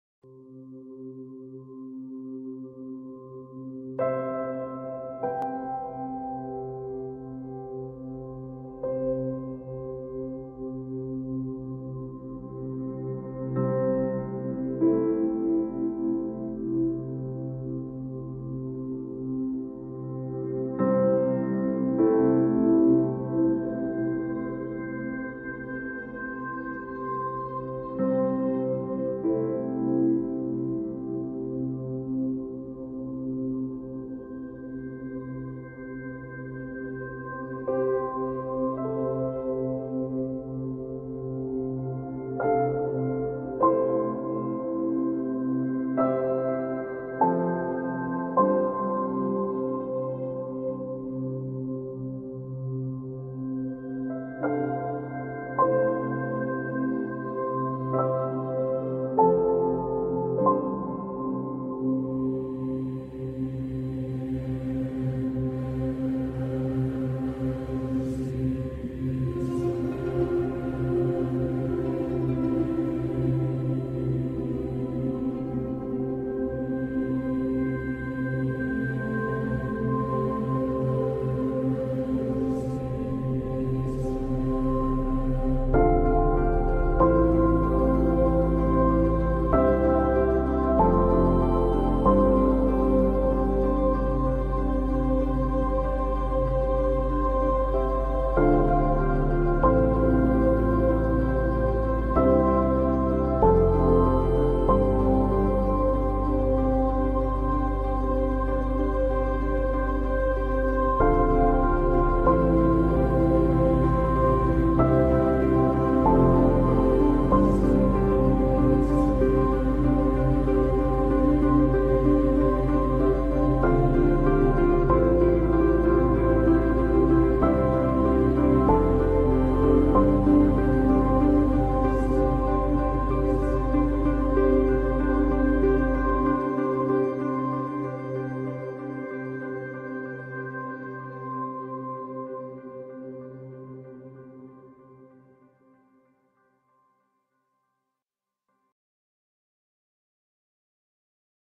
Nuuksio National Park, in Autumn.
Aerial footage of Nuuksio National Park, Espoo, Finland